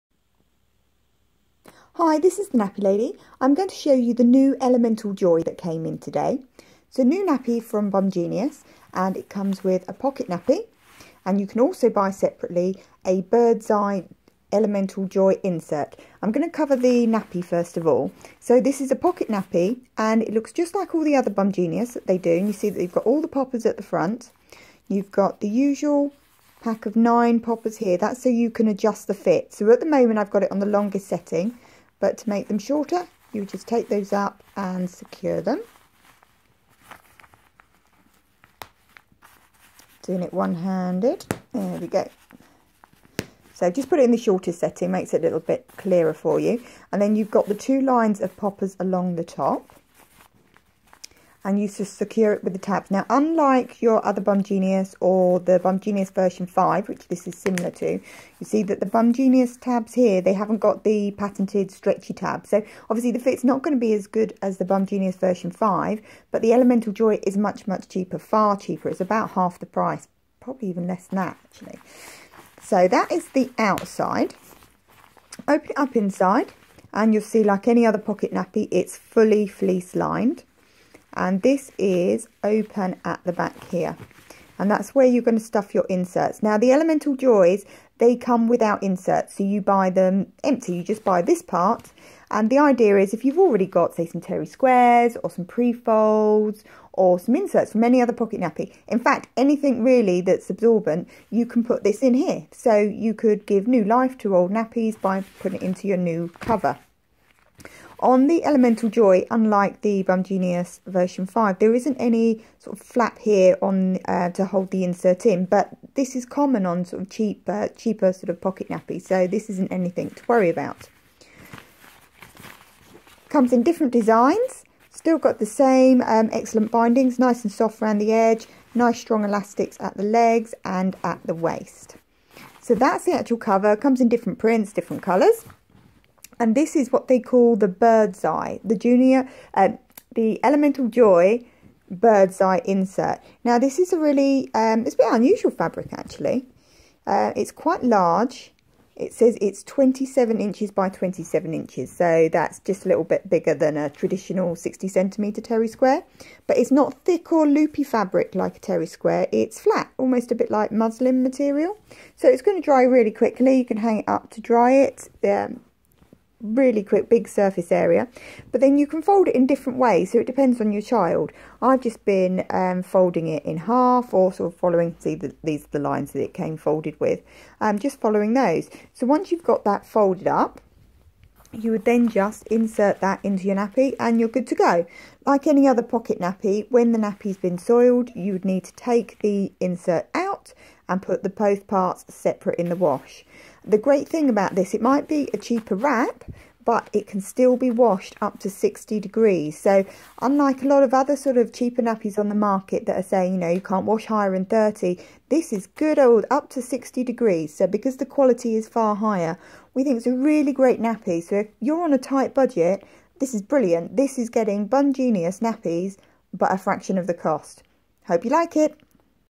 ElementalJOY Pocket Nappy Video Guide - The Nappy Lady
We get a closer look of the Bumgenius ElementalJOY, a value pocket nappy by Bumgenius, in this video guide by The Nappy Lady.

If you are new to cloth and don't know where to start, we can help

If you are experiencing problems using your nappies, we can support you